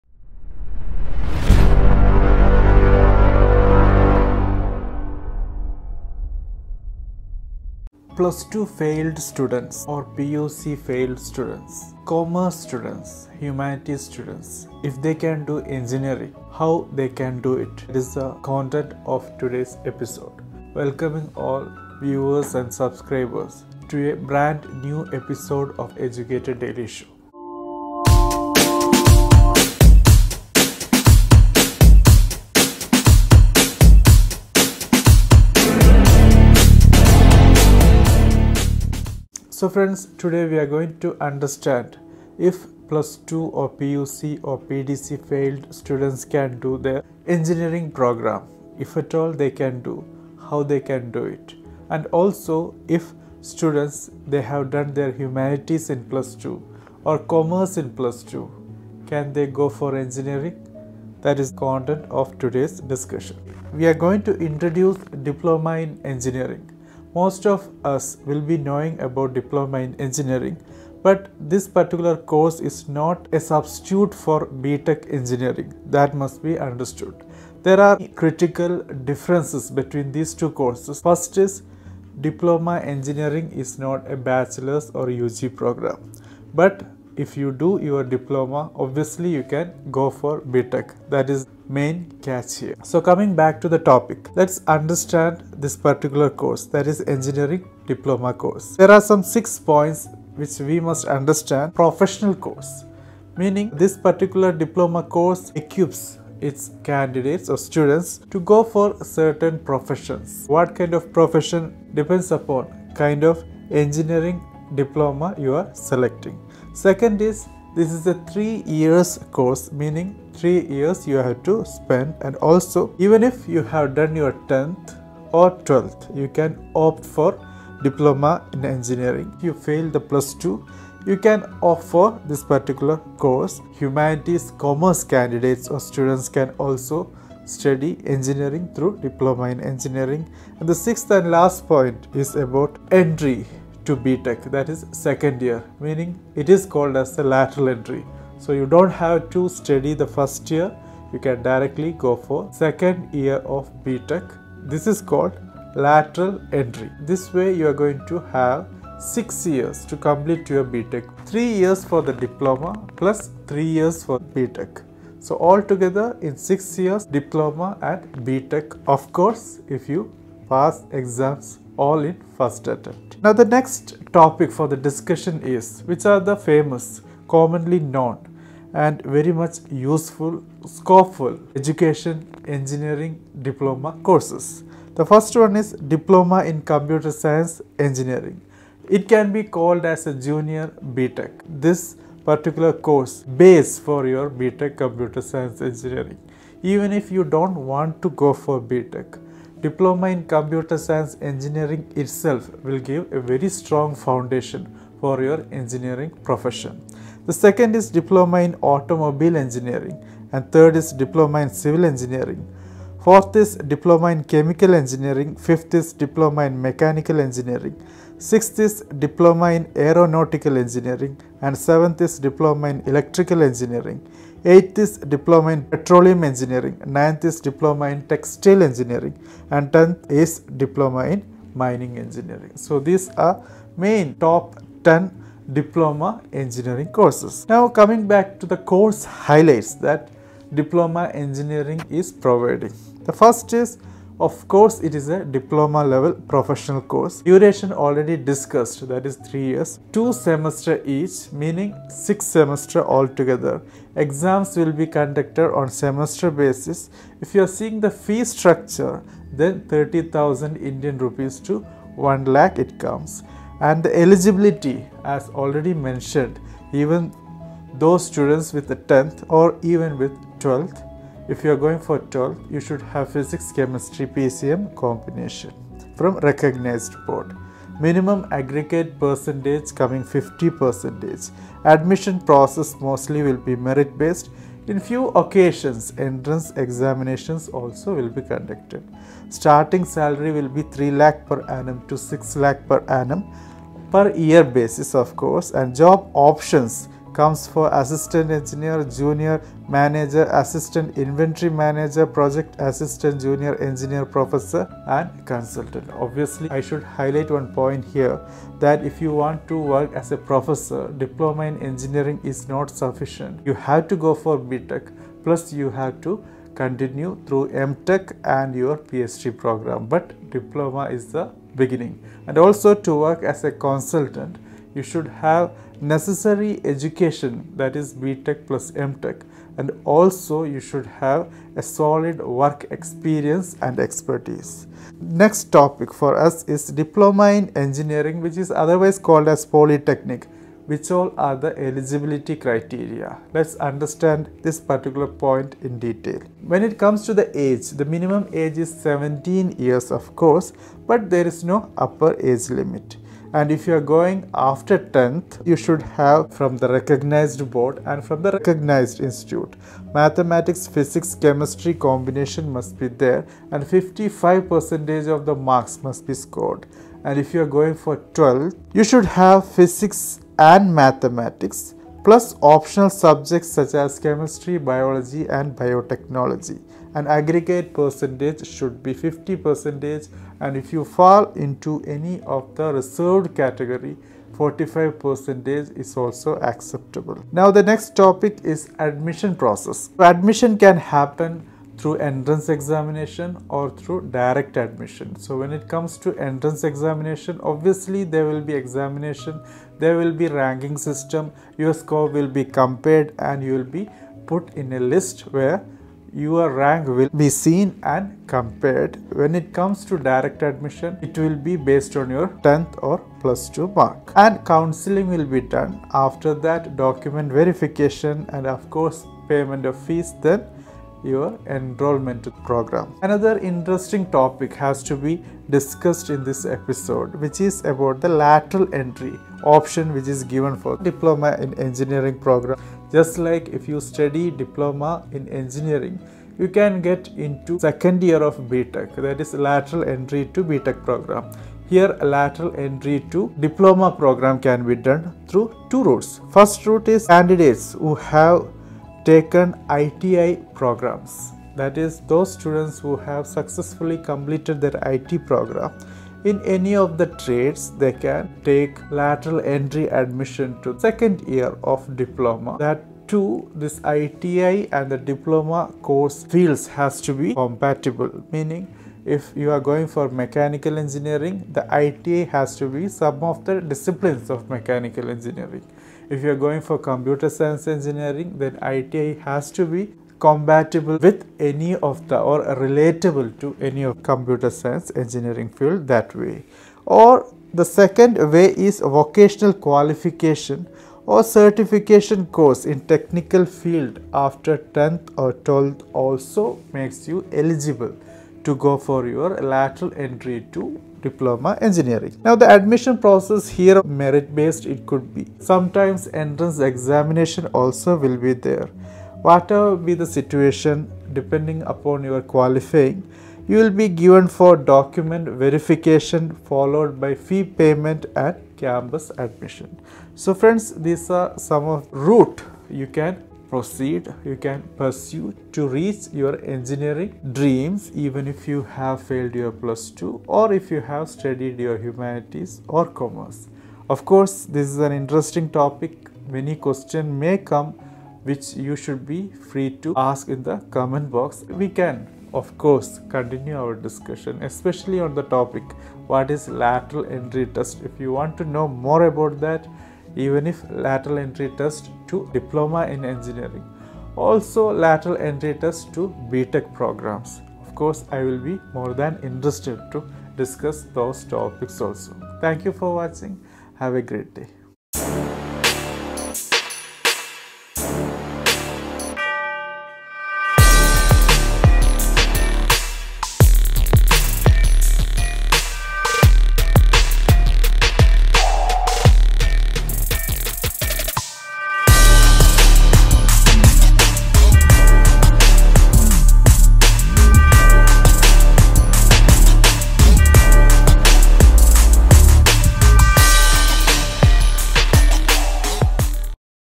+2 Failed? Humanities/Commerce Studied? You can become Engineer!
You can do Engineering after failing plus two, after tenth, for humanities and commerce students.

Engineering Diploma Courses (Polytechnic):

A professional course
3 years course
Can do after 10th or 12th
Can do even if you fail plus two
Can be done by humanities/commerce students
You can complete B tech in 6 years (3 years diploma and 3 years B Tech)

Famous Diploma Courses:

Diploma in Computer Science Engineering
Diploma in Automobile Engineering
Diploma in Civil Engineering
Diploma in Chemical Engineering
Diploma in Mechanical Engineering
Diploma in Aeronautical Engineering
Diploma in Electrical Engineering
Diploma in Petroleum Engineering
Diploma in Textile Engineering
Diploma in Mining Engineering

Course Highlights:

Level : Diploma Level
Duration : 3 years (6 semesters)
Fee Structure : INR 30K- 1 lakh
Exam Type : Per Semester
Eligibility : Must pass 10th or 12th in PCM from a recognised board
Minimum Aggregate Required : 50%
Admission Process : Merit-Based, Entrance Exam
Starting Salary : INR 3 to 6 LPA
Job Options : Assistant Engineer, Junior Manager, Assistant Inventory Manager, Project Assistant, Junior Engineer, Professor, Consultant, etc.

Diploma in Engineering (Polytechnic) Eligibility Criteria

Age Limit : Minimum of 17 years of age at the time of applying for the admission. No upper age
Education Qualification

After 10th: Must pass class 10th with main subjects as Mathematics, Physics and Chemistry from a recognised board, aggregate required of 55%

After 12th: Pass class 12th from a recognised board, 12th major subjects as Mathematics and Physics and one of the optional subjects as Chemistry/ Biology/ Biotechnology, aggregate 50% (45% for the reserved candidates).

Admission Process:

Entrance examination/Direct Admission - if qualify, then counselling process - document verification - pay fees - start program

Lateral Entry in Diploma in Engineering (Eligibility and Admission Process)
The candidates can also apply for lateral entry to Diploma in Engineering through which they can get direct admission to the second year of Polytechnic.
Diploma in Engineering (Lateral Entry is for a total duration of 2 years).

Eligibility: A degree in ITI (technical field) from a recognized institute.

OR

A vocational qualification or a certification course in Technical Field after completing 10th/ 12th from a recognised board.

Admission Process: Merit based/marks got in the qualifying examination - documents verification - fee payment - campus admission

Presenter & Content created by: Abdul Mujeeb M S (Mujeeb Patla)

മുജീബ്‌ പട്ലയെ പരിചയപ്പെടാം.. | Mujeeb Patla - Introduction

The Educater
Educate . Empower